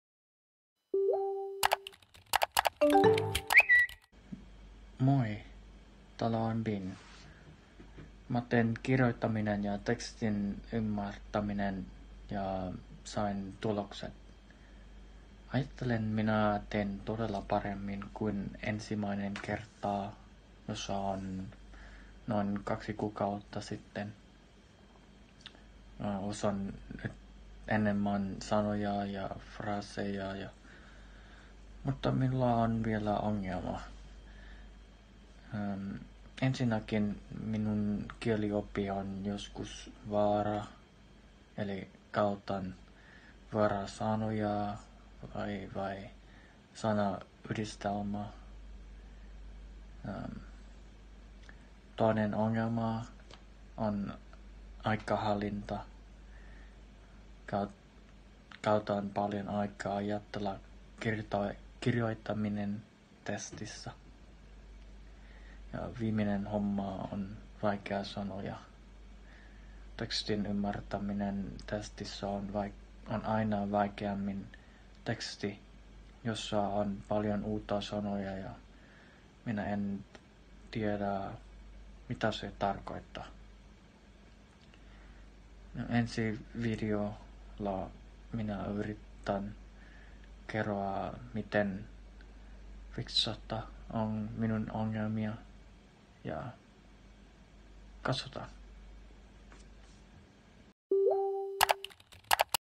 Binh #1 joulukuuta suomi online yki testi
Binh ilmoittautui seuraavaan YKI-testiin! Hienoa, Binh! Kuuntele Binhin tarina ja tsekkaa meidän YKI-sivut ja tule mukaan kurssille Gimaraan ! Binh signed up for the YKI test - well done Binh!